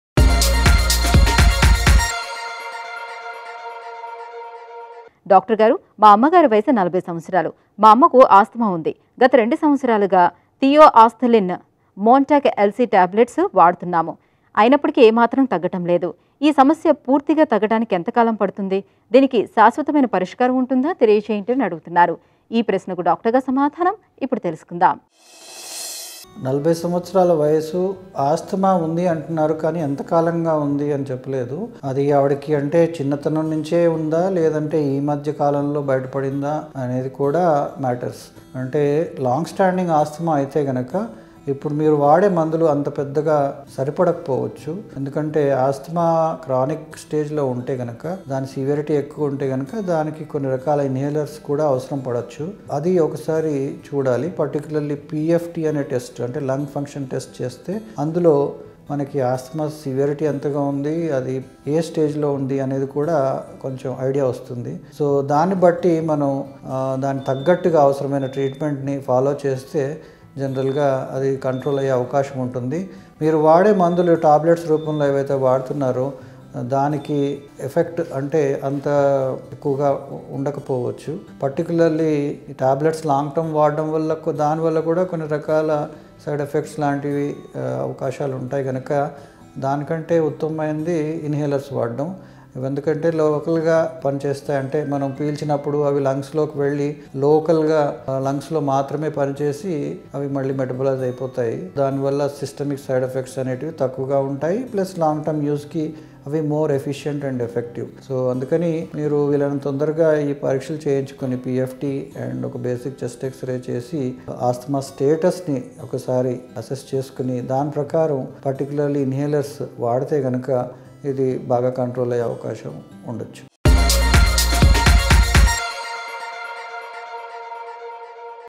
How Long Does It Take For Asthma To Cure Completely? | Dr.ETV | 23rd Dec 2022 | ETV Life
ఆస్తమా పూర్తిగా తగ్గడానికి ఎంతకాలం పడుతుంది?

Pulmonologist Dr T. L. N. Swamy addresses the viewer's query about the cure for asthma at 40 years of age, he clears misconceptions about medicines usage and presents his suggestions.